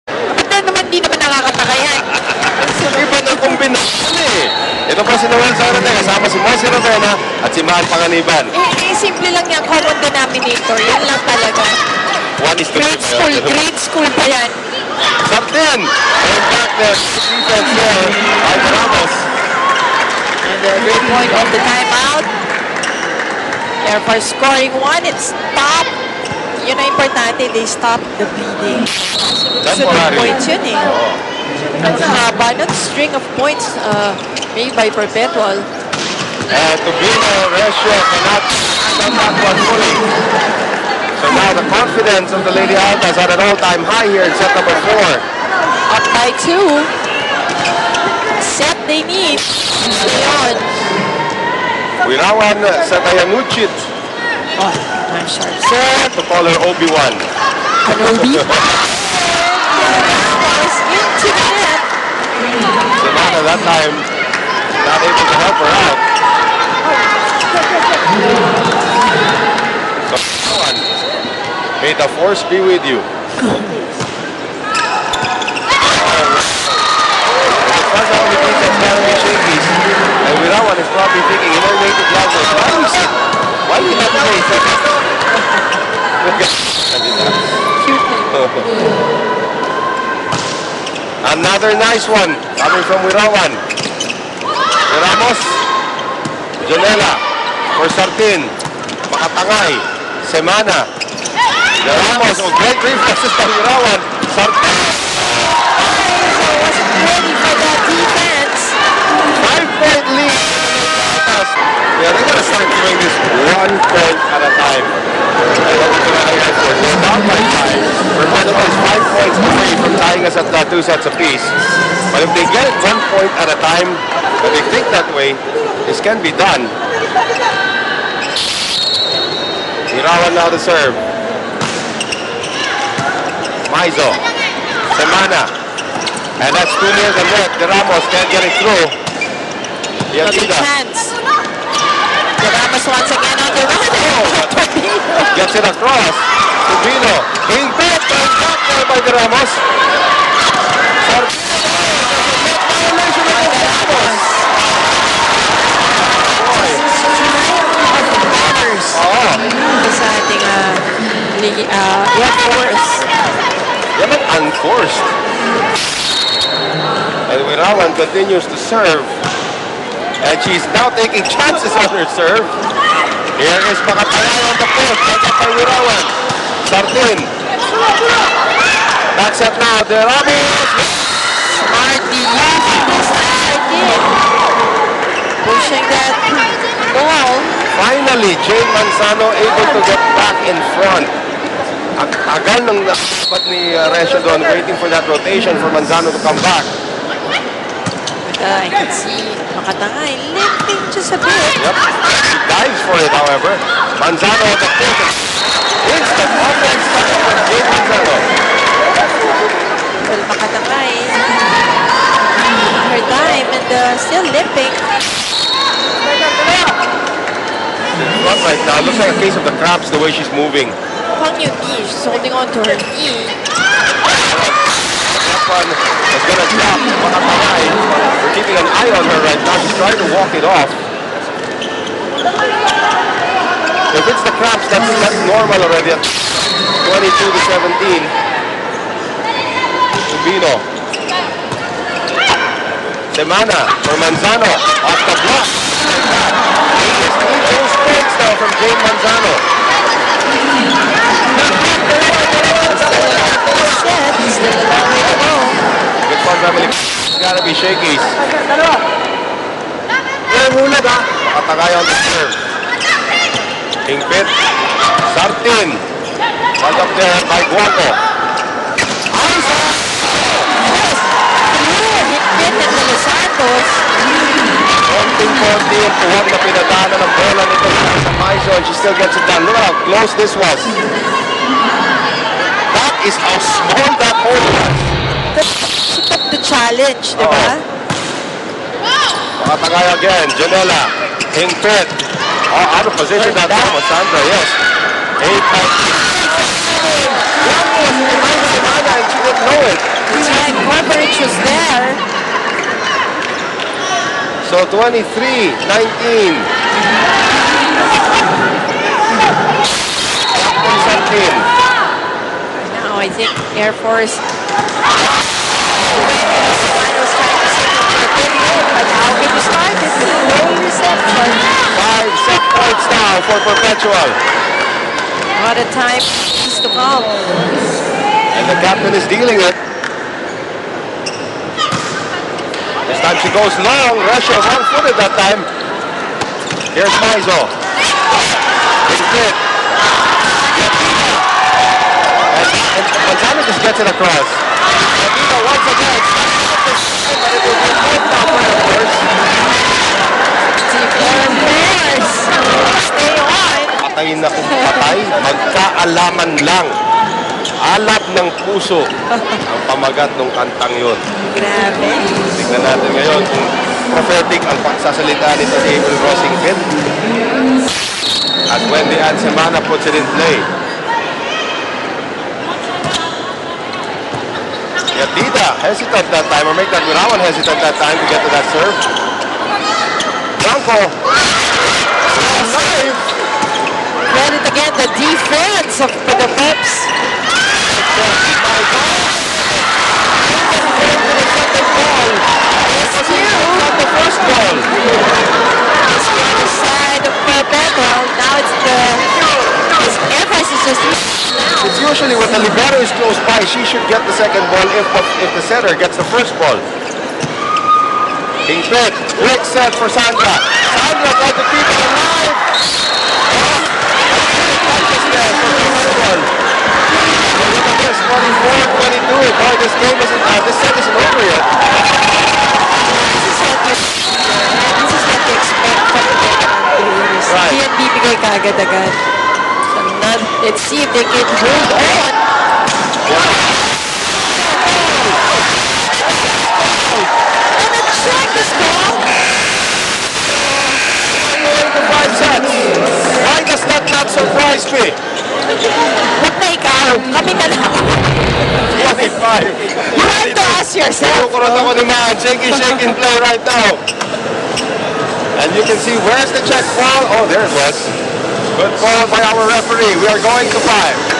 SVLeague-S8OC_AirForcevsPerpetual_PL - Set 4 (#2)
NOTE: Comments are now going to be turned off due to the fact that the videos i post just end up mostly as "bangayans".

Preliminaries match between the Philippine Air Force and the Perpertual Lady Daltas...